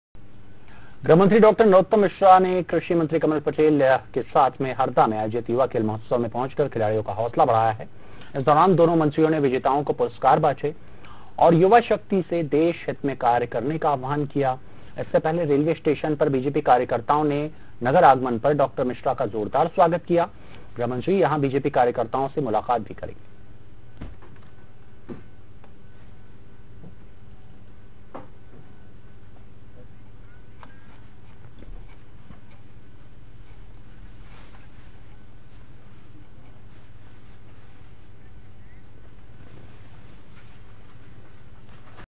खेल महोत्सव में शामिल होने हरदा पहुंचे गृह मंत्री डॉ नरोत्तम मिश्रा
Dr. Narottam Mishra Kamal Patel